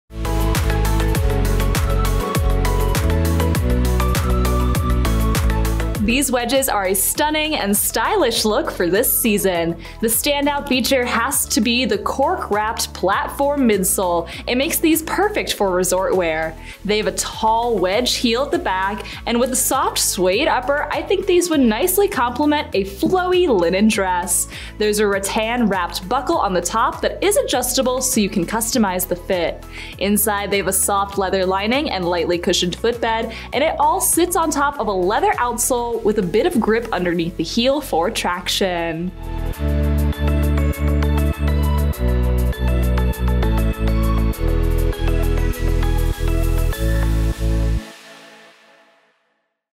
Veronica Beard Donata SKU: 9618190
Veronica Beard Women's Footwear Size Chart
Classy and elegant, the Veronica Beard&reg; Donata Wedge Heels are a perfect statement-maker.
Suede upper.
Leather lining and insole.
Slip-on style with a buckled vamp.
Open almond toe.
Synthetic outsole.
Imported.